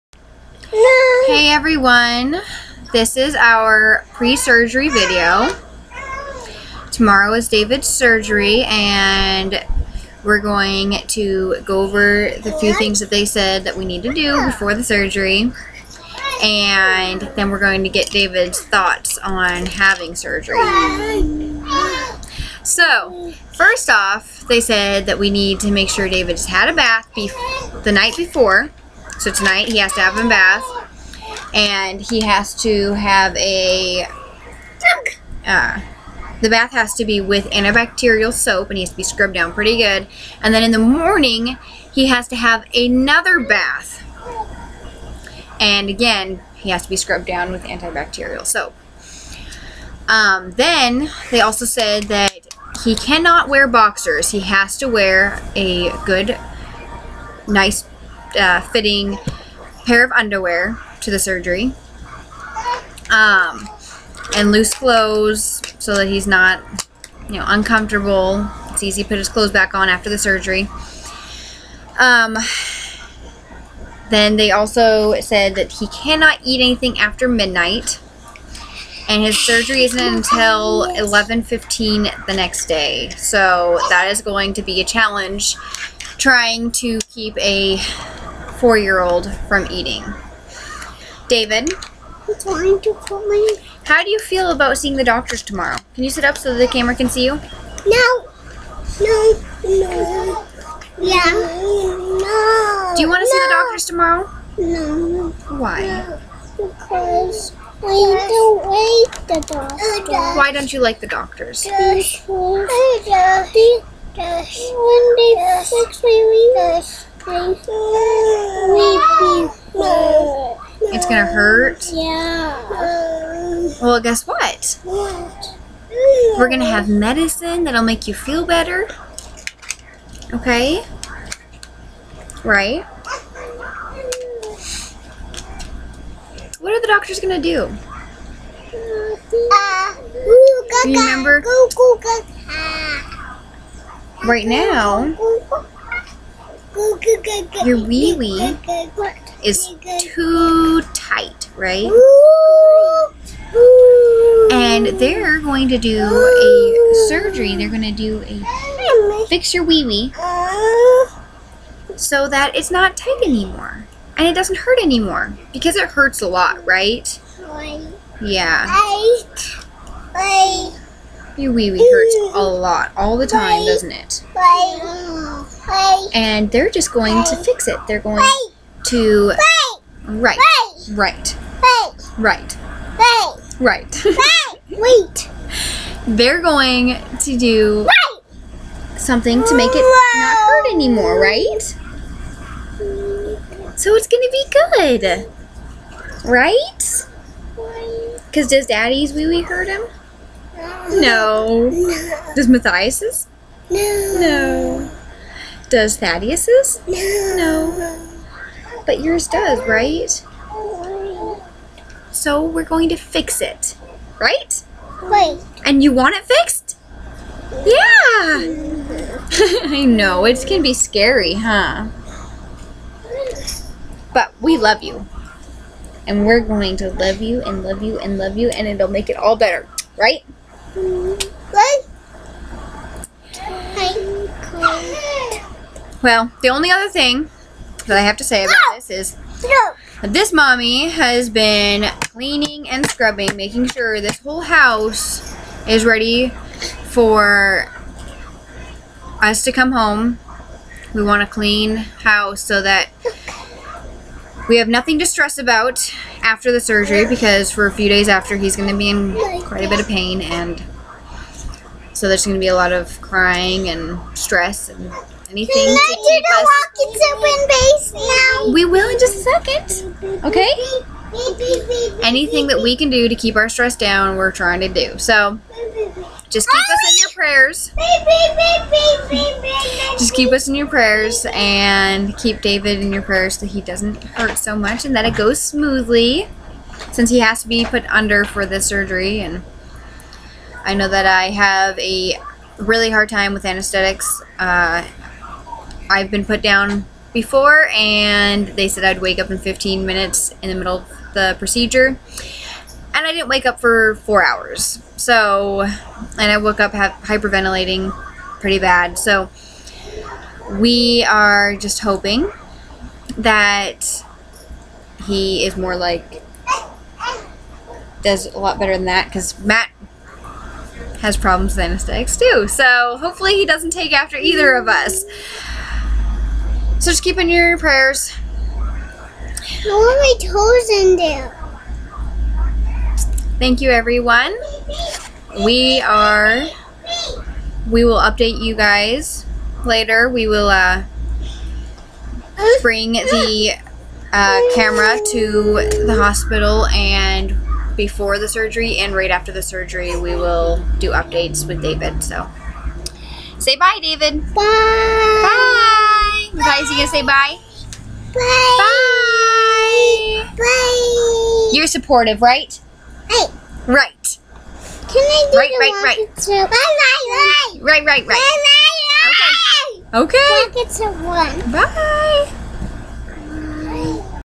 Davids "the day before" pre-surgery video.
David is having surgery tomorrow and we are asking for prayer and support for him.